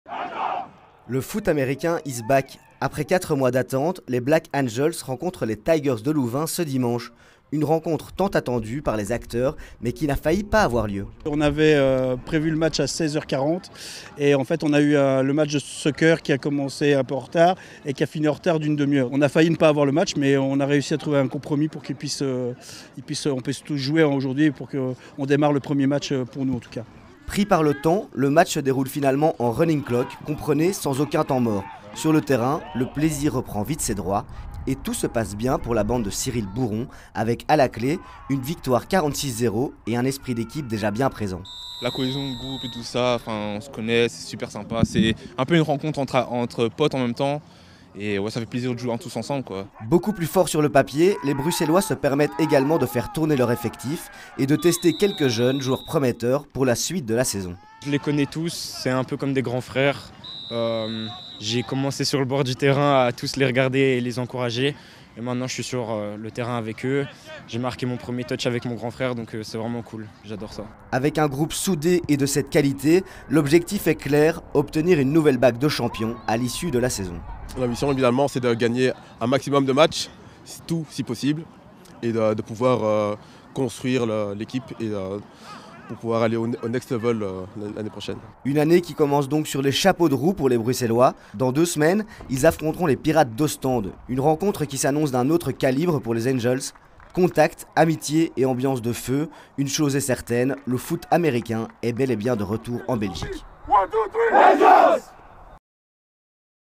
Foot américain : les Black Angels commencent bien leur saison face à Louvain
Pour capter BX1 en télévision, à Bruxelles et en Wallonie :
Proximus : Canal 10
Telenet : Canal 10
VOO et Orange  : Canal 61